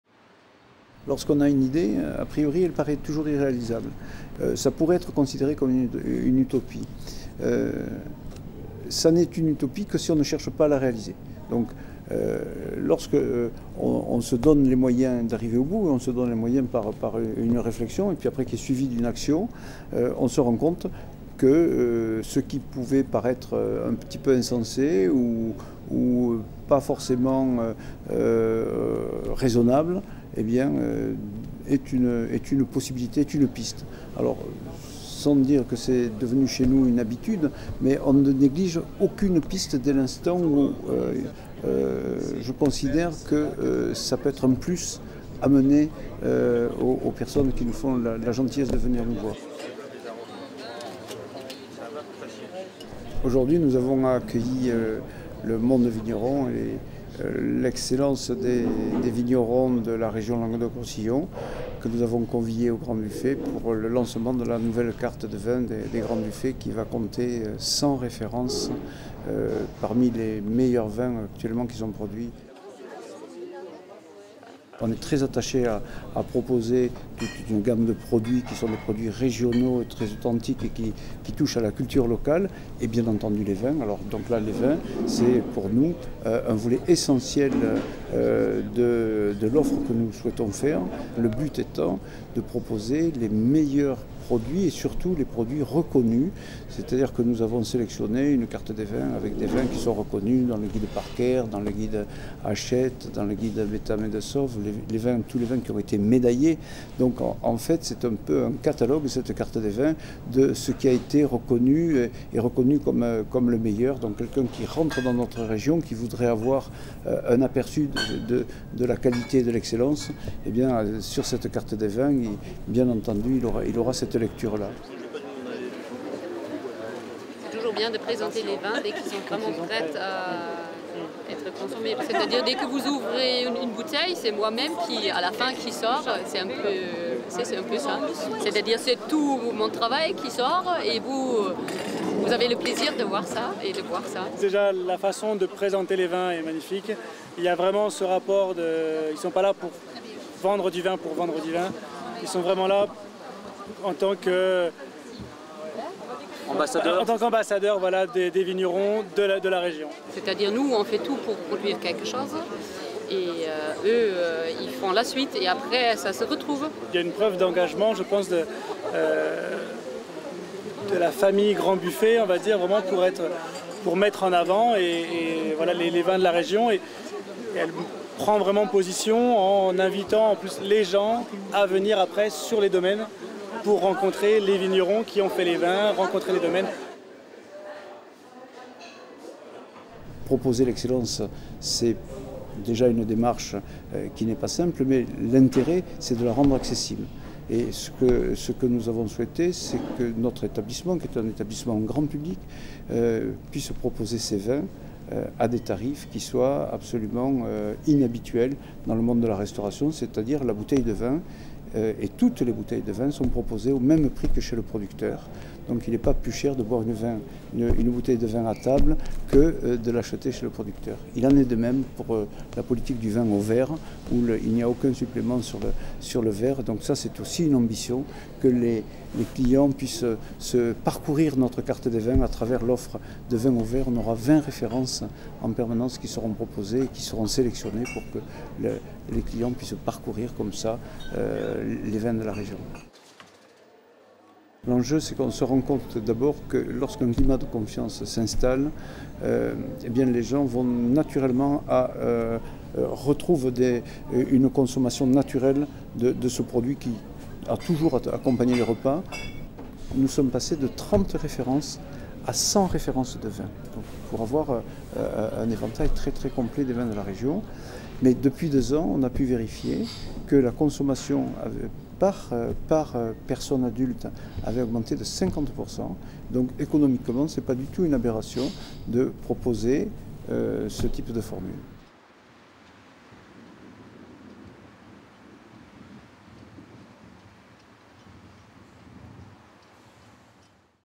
Les Grands Buffets : Vins du Languedoc-Roussillon
Louis PRIVAT, présente la gamme des vins du Languedoc Roussillon qui seront désormais disponibles sur la carte du  restaurant des Grands Buffets de Narbonne.